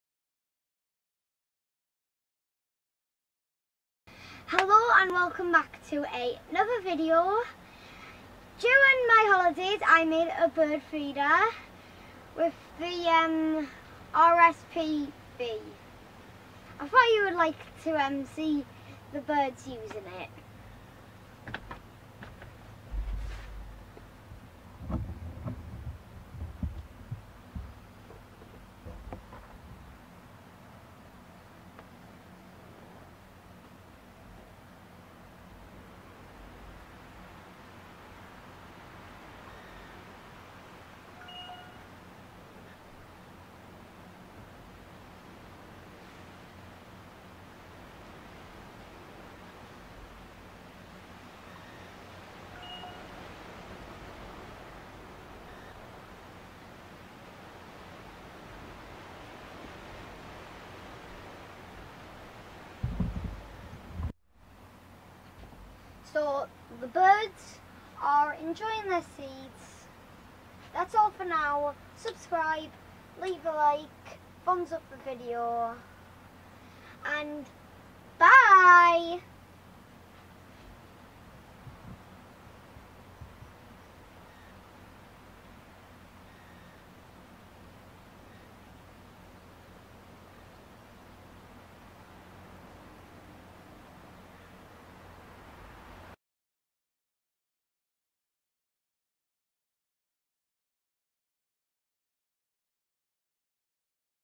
RSPB Bird feeder
During the holidays I made a bird feeder with Eleanor from RSPB Galloway